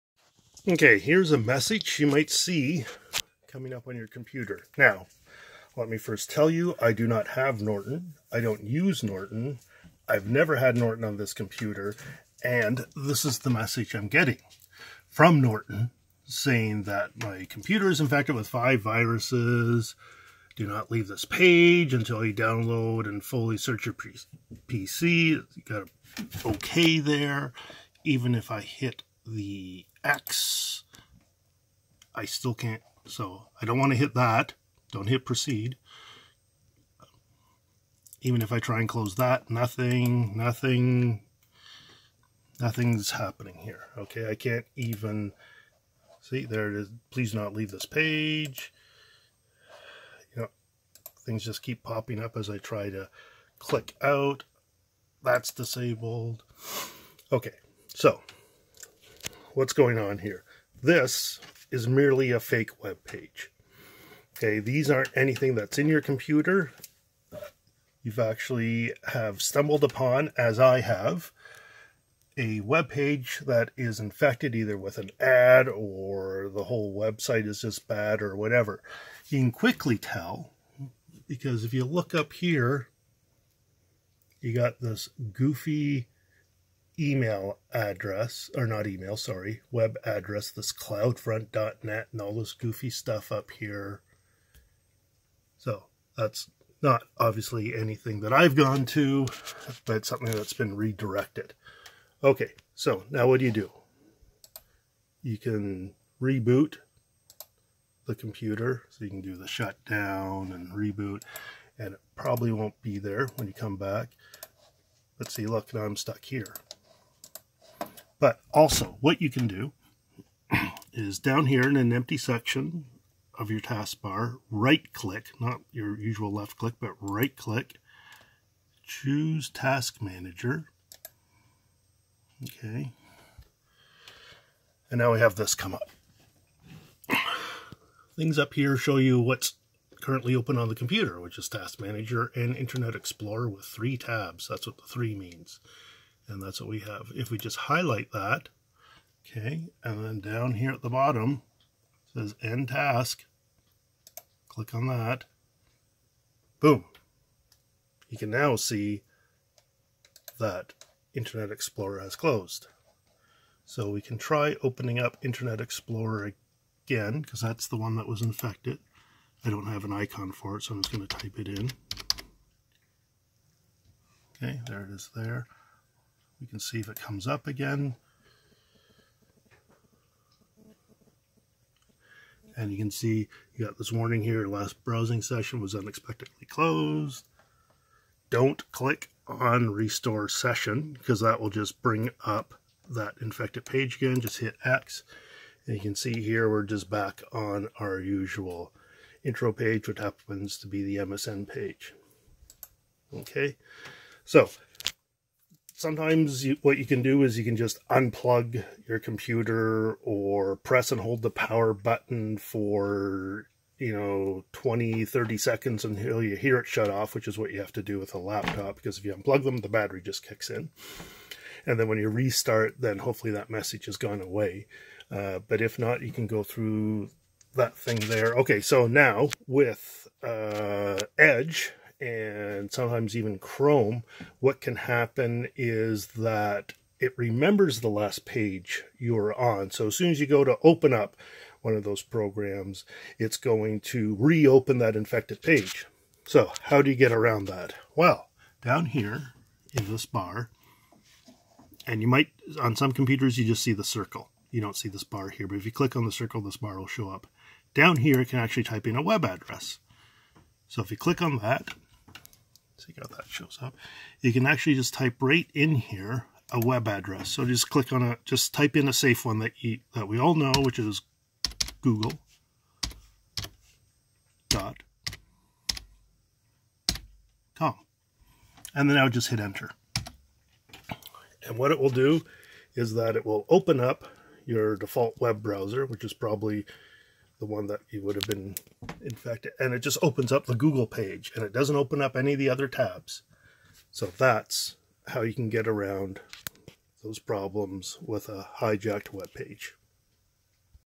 Fake Norton Antivirus Removal
Sometimes you might get a message on your computer alerting you of a bunch of viruses and you can't close the alerts.  This is a scam.  Here is how to recognize it and stop it.